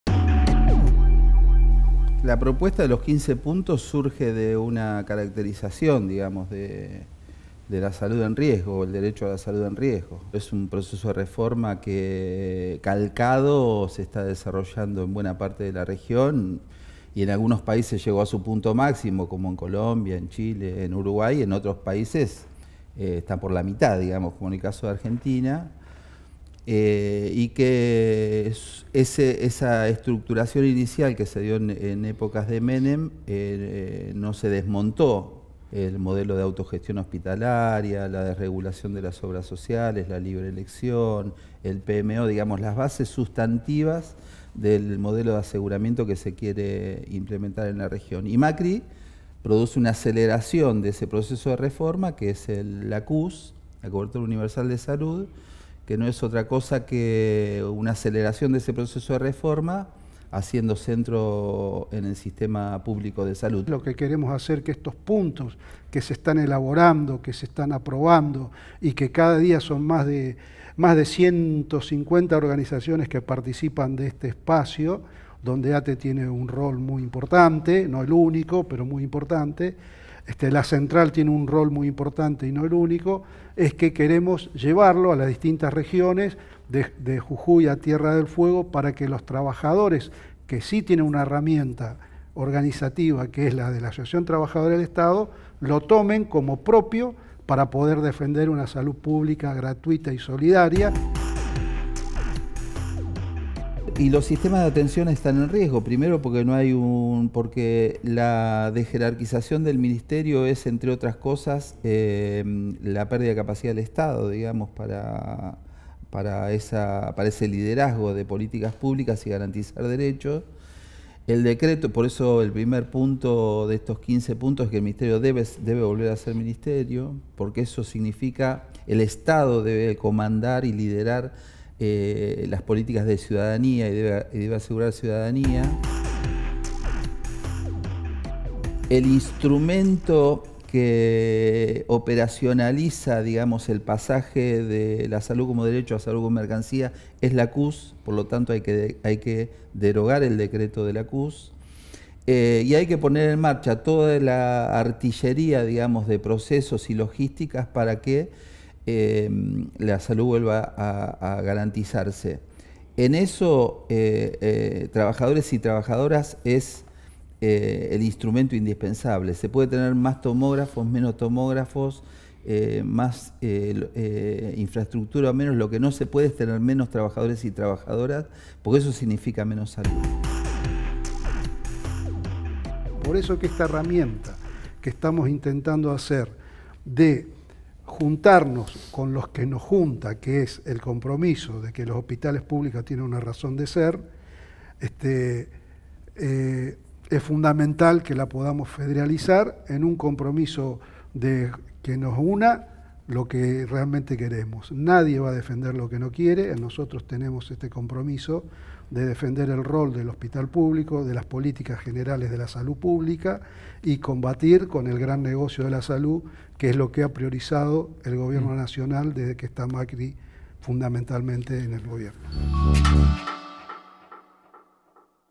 La agenda sanitaria del campo popular en el proceso electoral
Más de 100 organizaciones sociales y sanitarias redactaron 15 puntos base de un acuerdo para instalar la agenda sanitaria del campo popular en el proceso electoral. “Hay que revertir  el deterioro de las políticas públicas de Salud y que vuelva a ser Ministerio”, expresó Daniel Godoy, de IDEP-ATE.